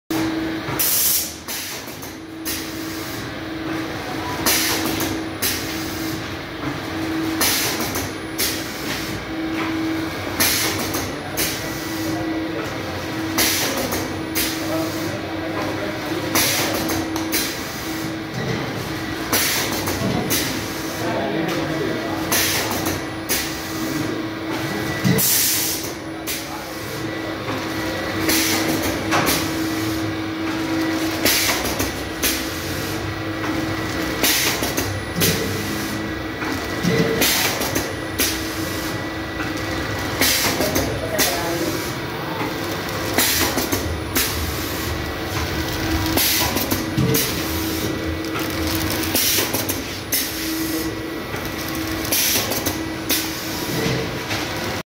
Pouch Packing Machine with Screw Conveyor
Packing Capacity 100gms to 1kg Pouch
SS Screw Conveyor with Ribbon Blender type feeder
Techno Motion Engineering
Ahmedabad